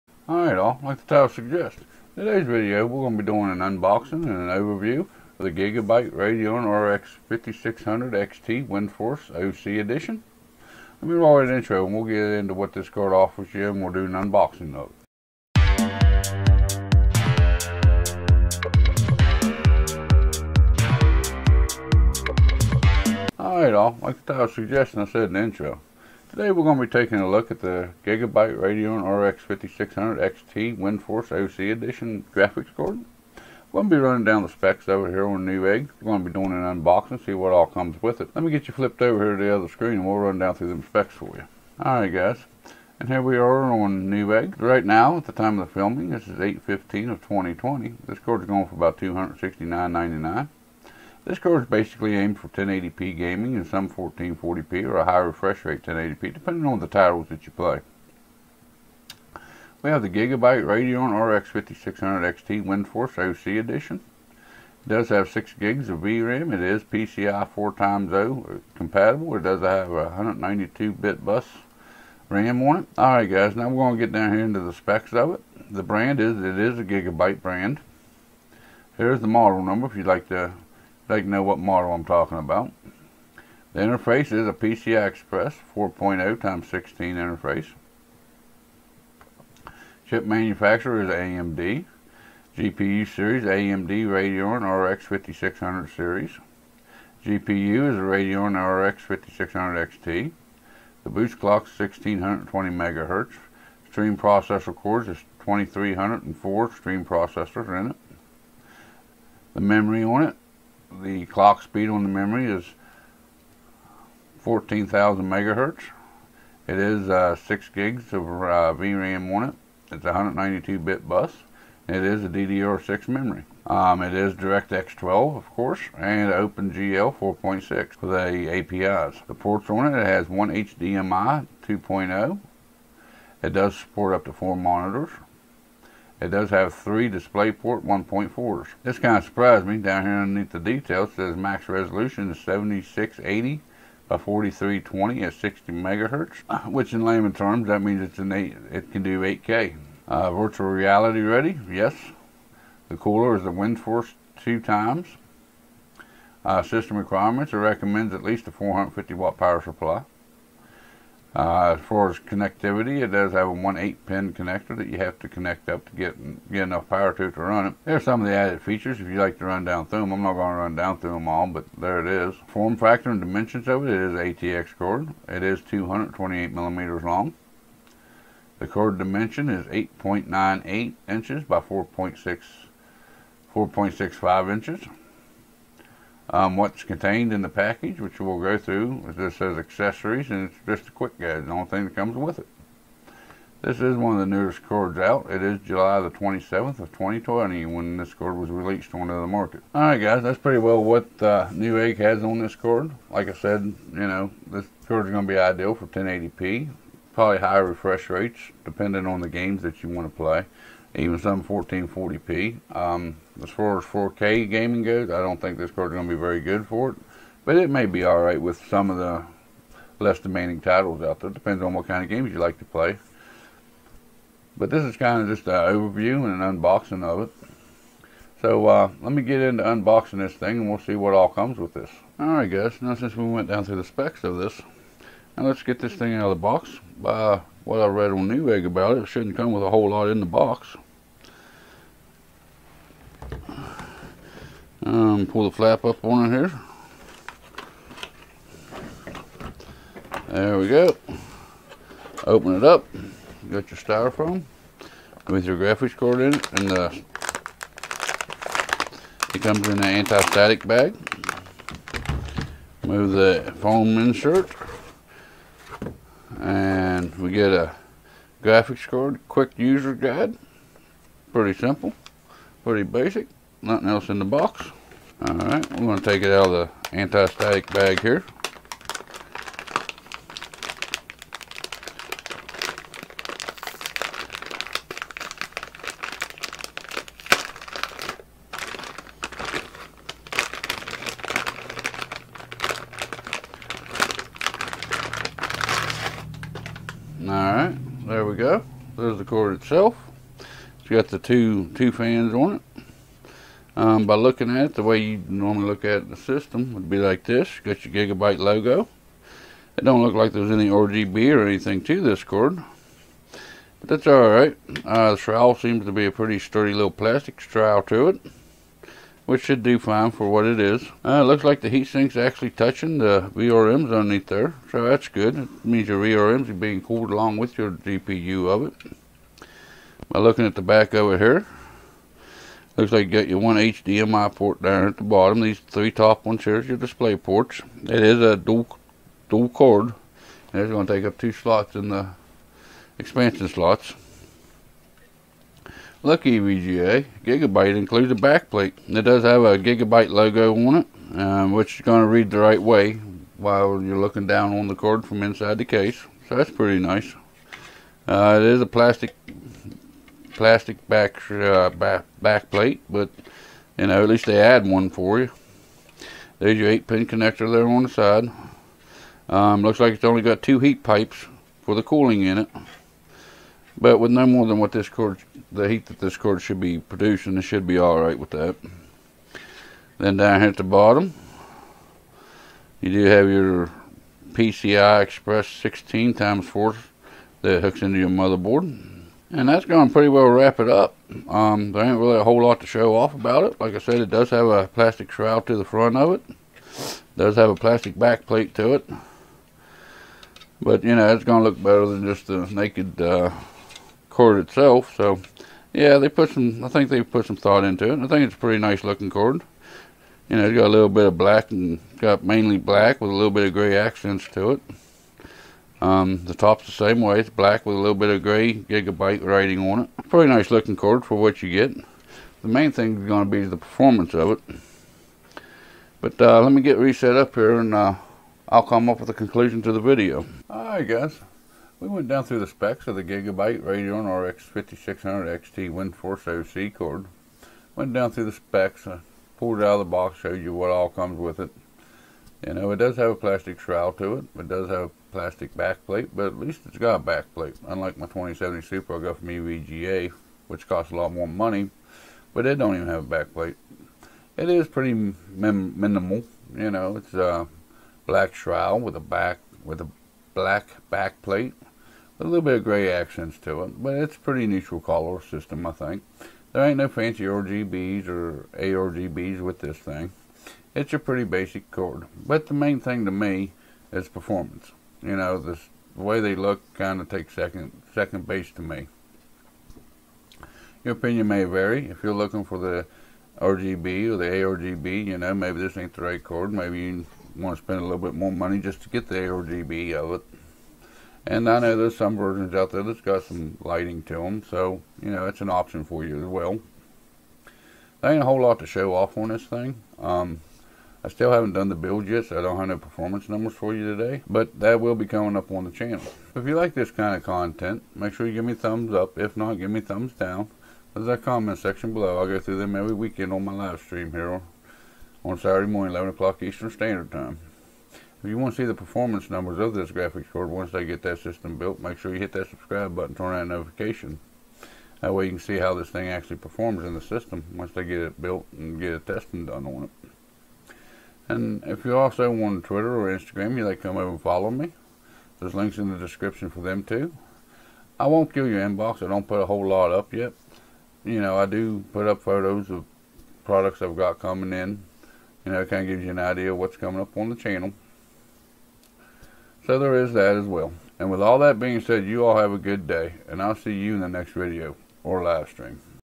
GIGABYTE Radeon RX 5600 XT WINDFORCE OC Unboxing and Overview
GIGABYTE Radeon RX 5600 XT WINDFORCE OC Unboxing and Overview, In this video we will be taking a look at the specs of the GIGABYTE Radeon RX 5600 XT WINDFORCE OC, and an unboxing to see what comes with it.